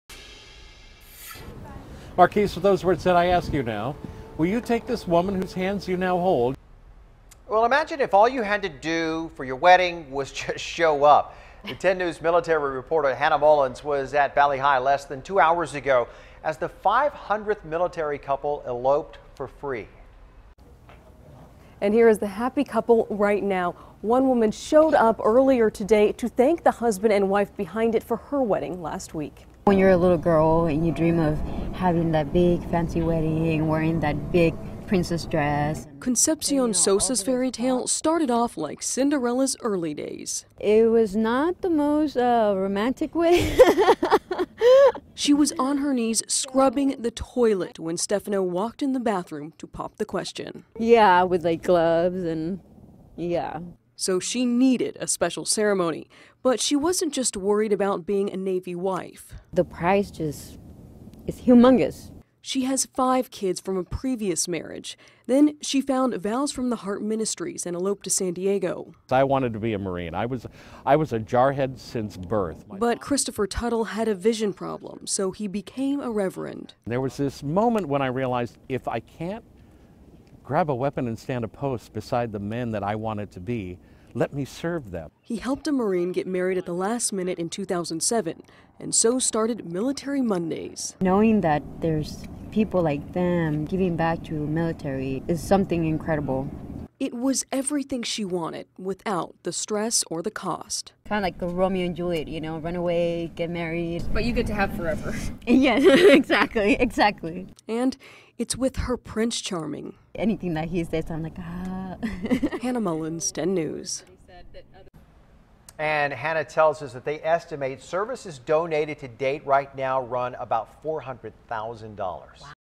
500th Military Mondays couple just got married
The free ceremony was held at the Bali Hai.